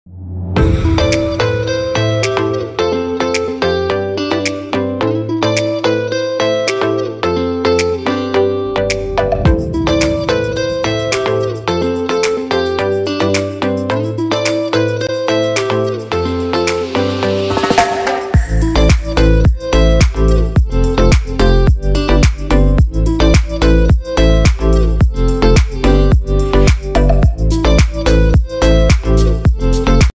Você não pode ser qualquer coisa que desejar ser. Mas pode ser tudo o que Deus quer que você seja.
Oi minha gente!
hoje é dia:08 de fevereiro de 2024
39 de 366 dias.

A frase de hoje é para refletir.

Músico: @iksonmusic